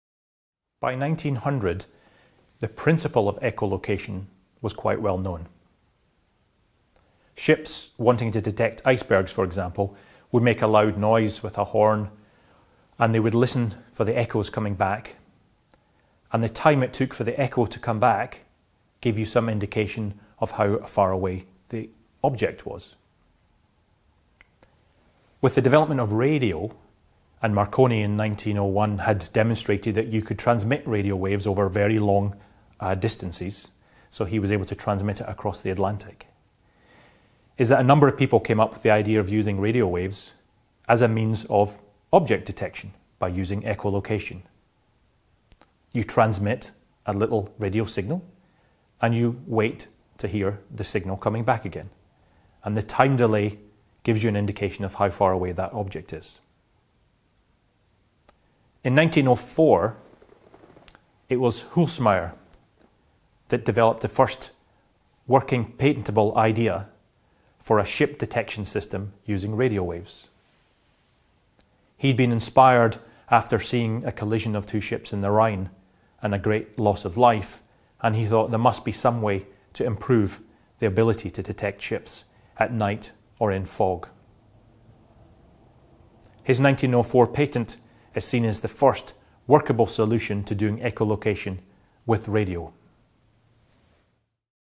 ESA Echoes in Space History: echolocation
By the turn of the century, the concept of echolocation was well known. A whistle or horn could be used to transmit a sound pulse, for instance, and the time delay to the echo used to determine the distance to an object. In 1900 Nikola Tesla (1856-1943), a Serbian-American inventor obviously aware of this technique, conceived of the possibility of employing radio waves not only to detect, but also to measure the movement of distant objects, although he never worked out the details of his idea.